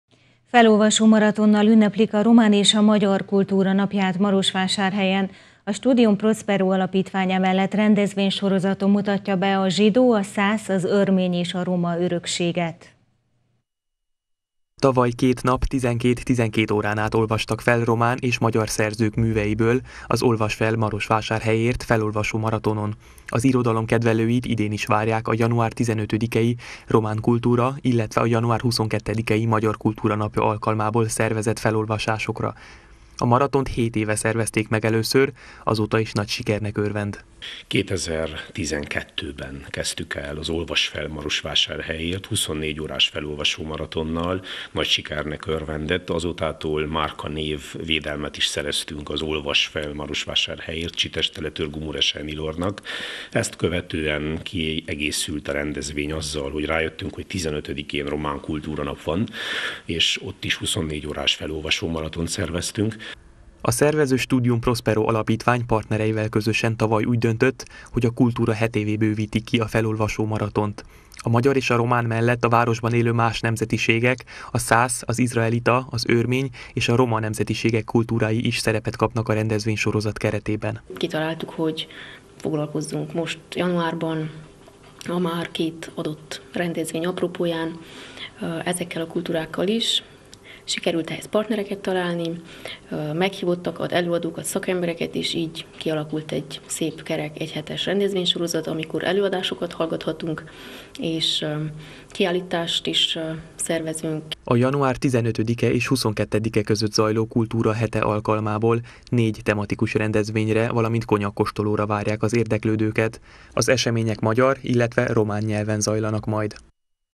Olvass fel Marosvásárhelyért! – Erdélyi Magyar Televízió
Felolvasómaratonnal ünneplik a román és a magyar kultúra napját Marosvásárhelyen. A Studium-Prospero Alapítvány emellett rendezvénysorozaton mutatja be a zsidó, szász, örmény és roma örökséget.